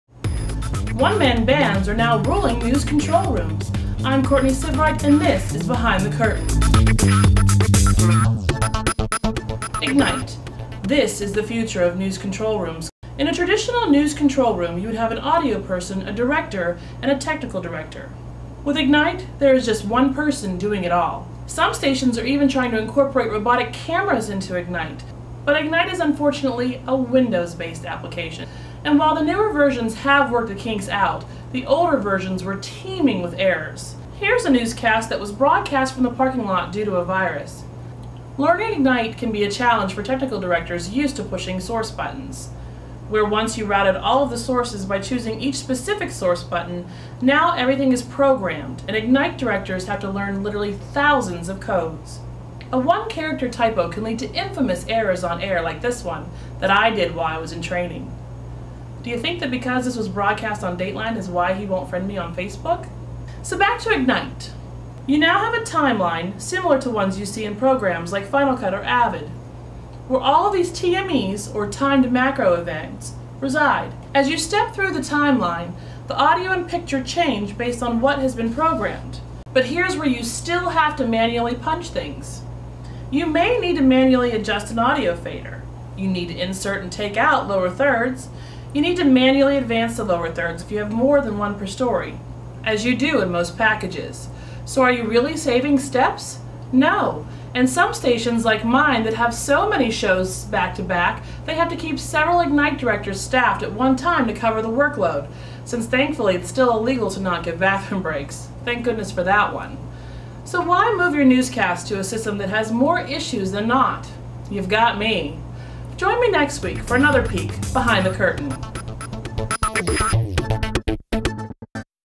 Ignite in a nutshell
This podcast shows the difference between traditional technical directing and switching a newscast on Grass Valley Ignite.

Here's the error I am referring to...